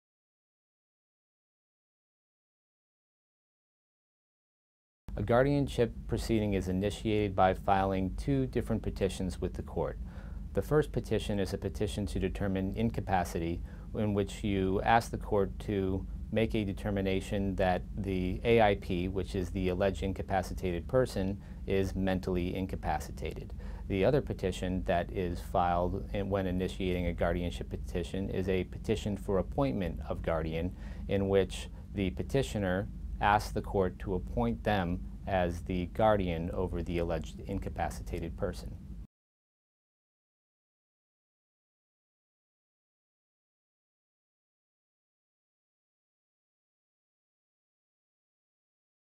How is a guardianship proceeding initiated?
How is a guardianship proceeding initiated? 312 Boca Raton, Florida 33431

A guardianship proceeding is initiated by filing two different petitions with the court. The first petition is a petition to determine incapacity in which you ask the court to make a determination that the AIP which is the alleged incapacitated person is mentally incapacitated. The other petition that is filed and when initiating a guardianship petition is a petition for appointment of guardian in which the petitioner asks the court to appoint them as the guardian over the alleged incapacitated person.